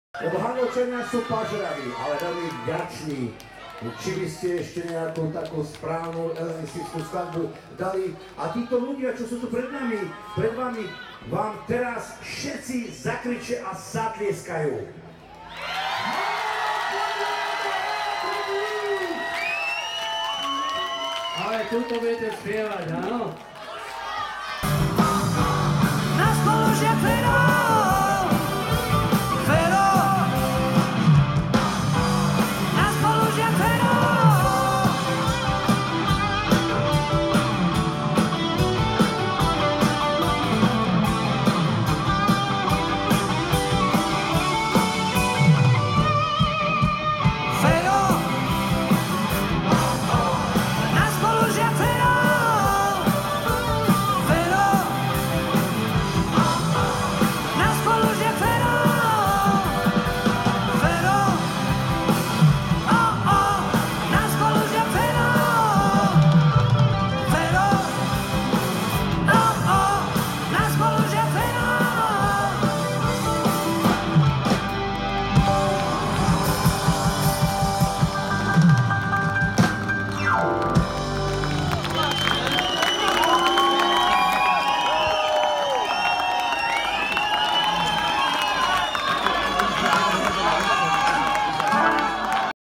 Elán Tribute v Handlovej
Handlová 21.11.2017

Tribute band (Elán Tribute) je typ hudobnej skupiny hrajúcej prevzatý repertoár inej skupiny (Elán). Jedná sa o hudobníkov s pozitívnym vzťahom k skupine, ktorí s obľubou interpretujú jej repertoár, pričom sa jej tak snažia vyjadriť svoju poctu.
Našim cieľom je hrávať zásadne živé koncerty, na ktorých je autentická a neopakovateľná atmosféra!